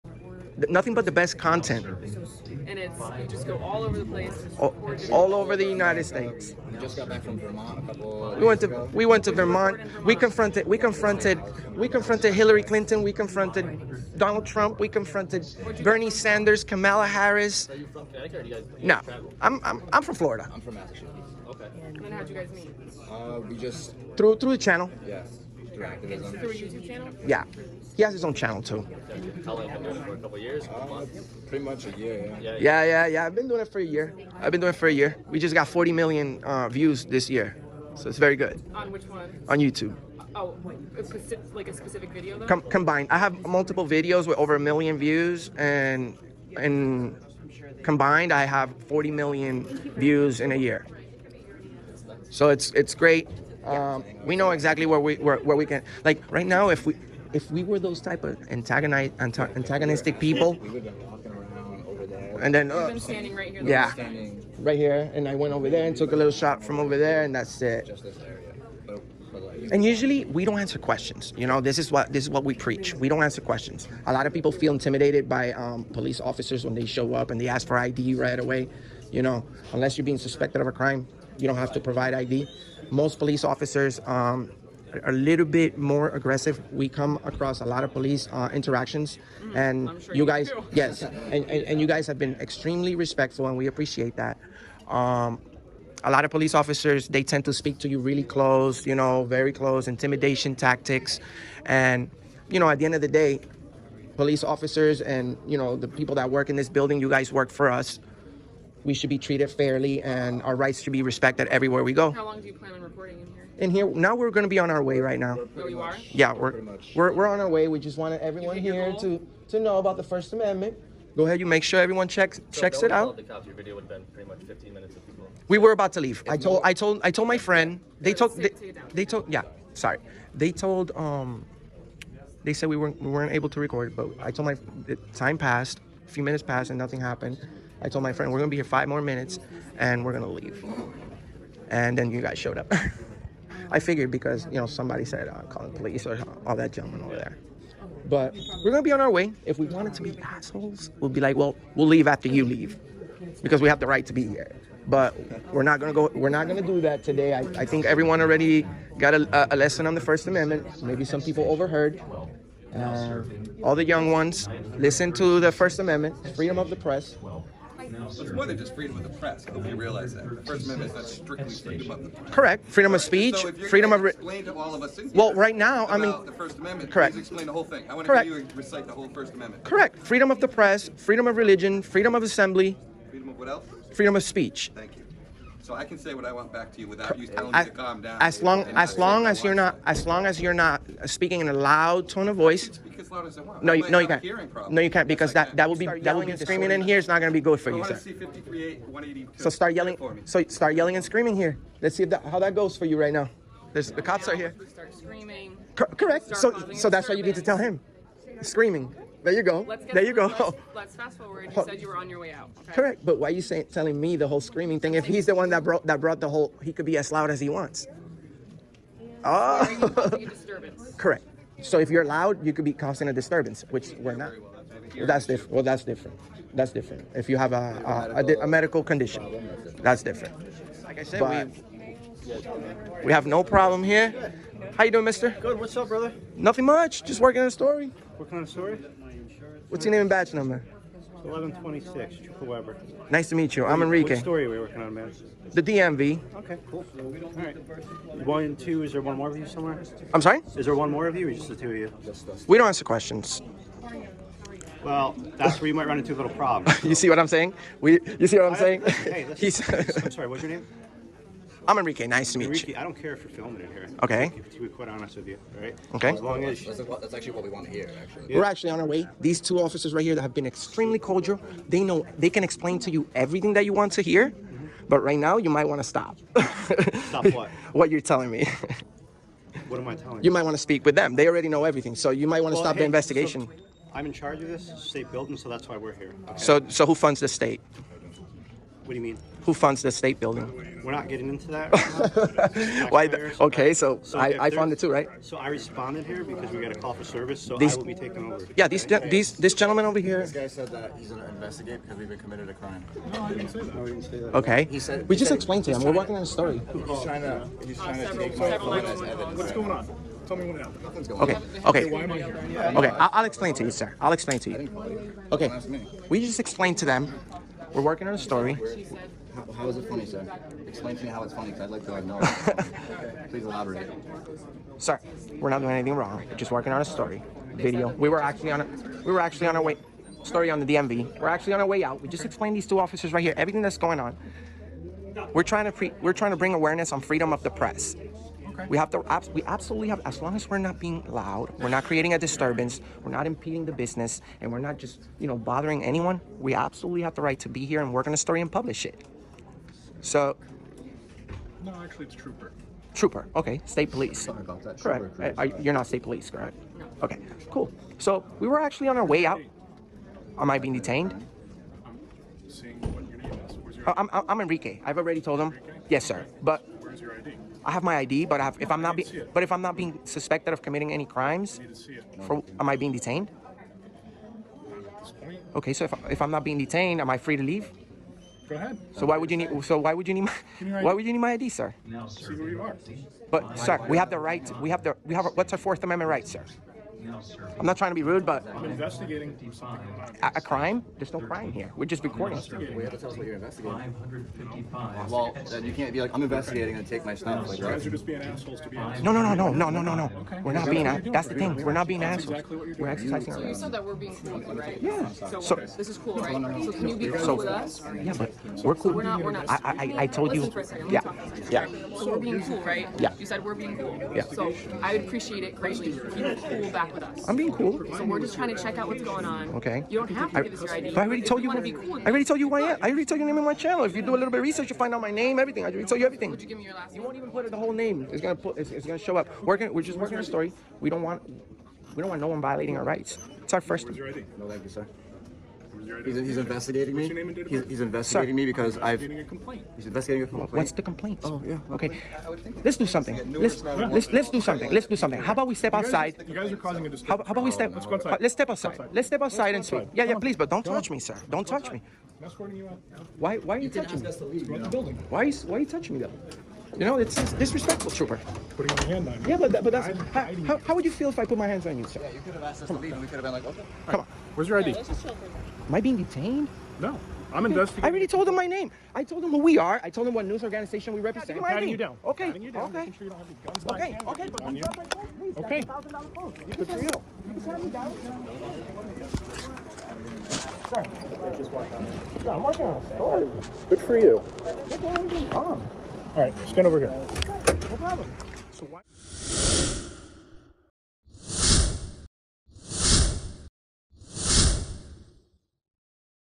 ONE OF THE WORST POLICE FAILS ON YOUTUBE! TYRANTS UNLAWFULLY REMOVED JOURNALISTS FROM DMV!
Mr @AuditingAmerica Was at a DMV..
This is how his Encounter went with the Fuzz.

I think it was pretty Disgusting how that last cop just escalated everything.